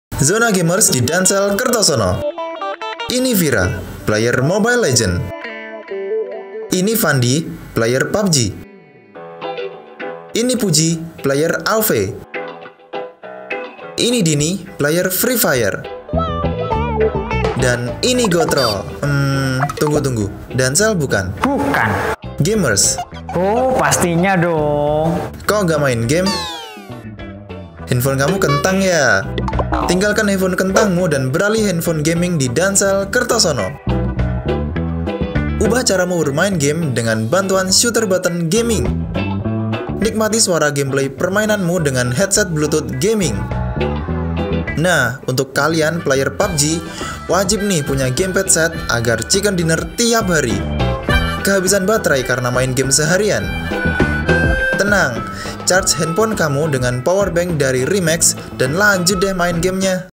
ZONA GAMERS !!! | DANCELL OFFICIAL
Ayo mabar bareng mas gotro di Dancell !!!   dengan asesoris pelengkap supaya main game nya lebih asik lagi dan bisa menang terus !!